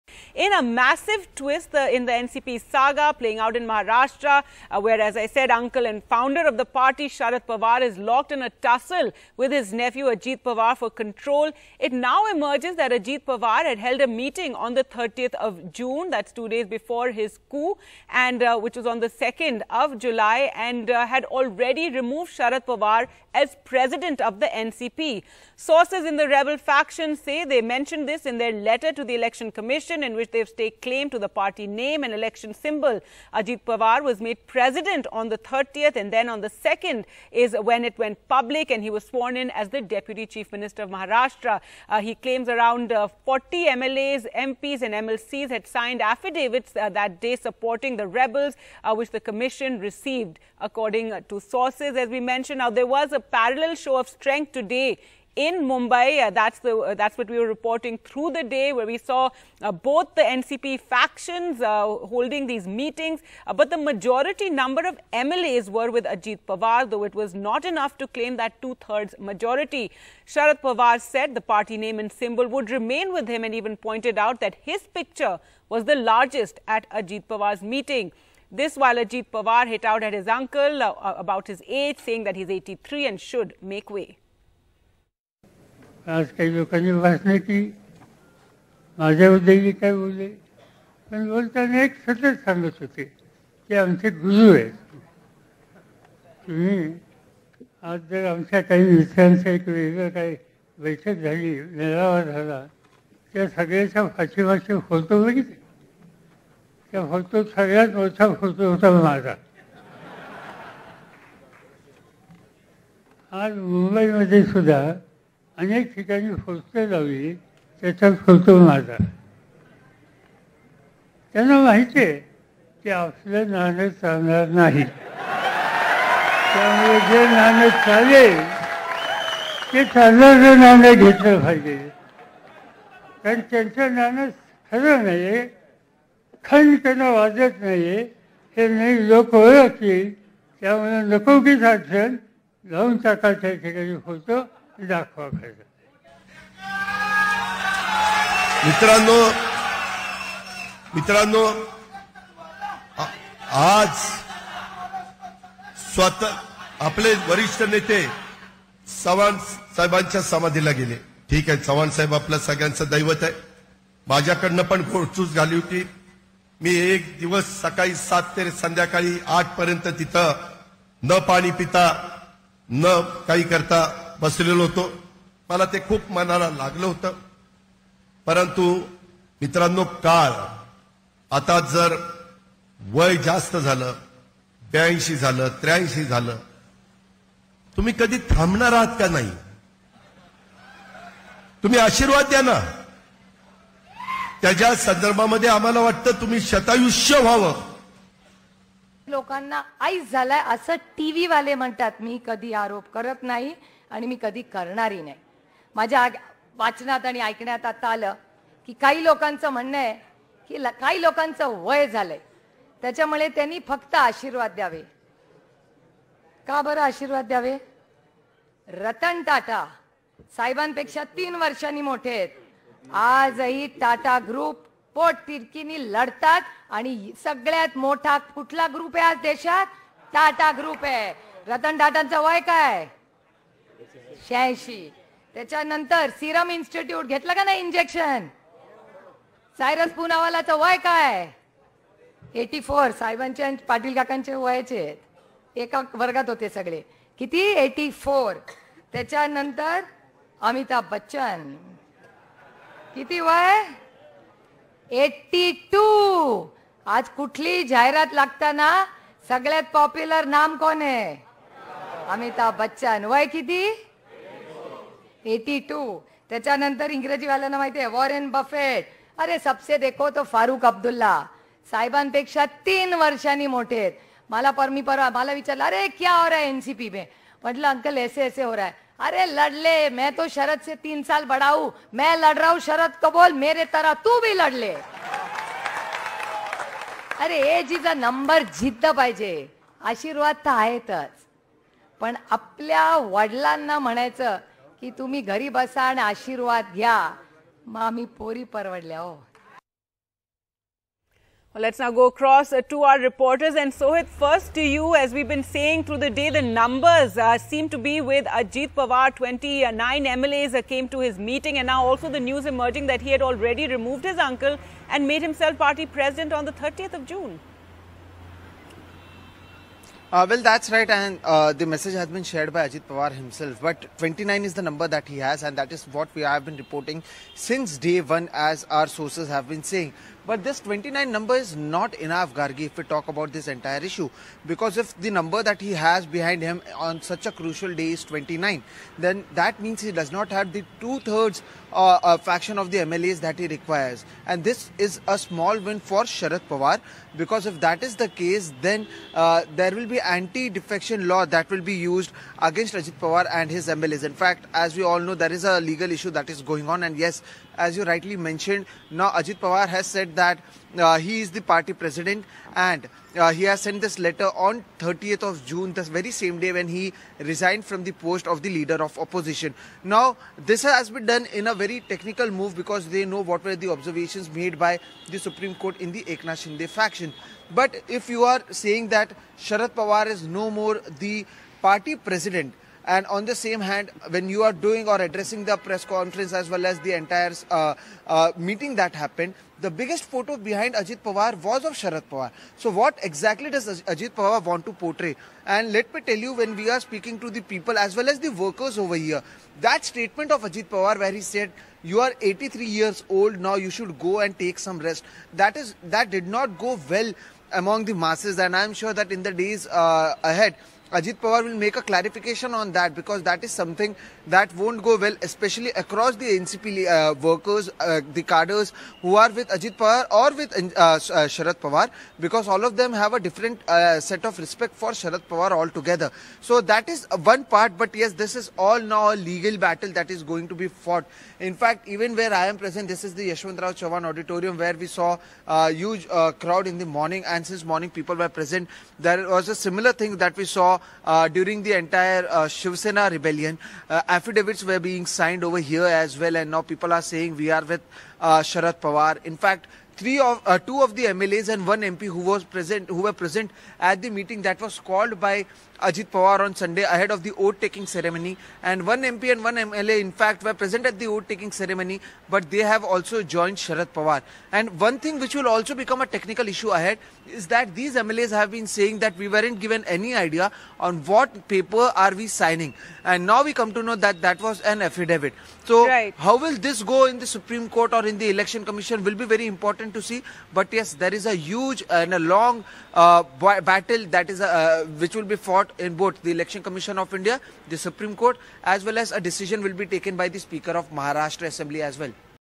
Ajit Pawar Says He Is NCP Chief, Not Sharad Pawar, After Show Of Strength | The News
Maharashtra Political Crisis: The rebel faction of the Nationalist Congress Party removed Sharad Pawar from the top post of the party he founded and led for over two decades, just two days before their mutiny. Ajit Pawar was elected the party chief by around 40 MLAs, MLCs and MPs -- a step calculated to negate any legal hurdle -- the Election Commission has been informed. Sources in the Election Commission said according to the rebels' letter, they had named Ajit Pawar as party president on June 30 -- days before his shock move of crossing over to the ruling alliance and taking oath as Maharashtra's Deputy Chief Minister. Around 40 MLAs, MPs and MLCs had signed affidavits that day supporting the rebels, sources said.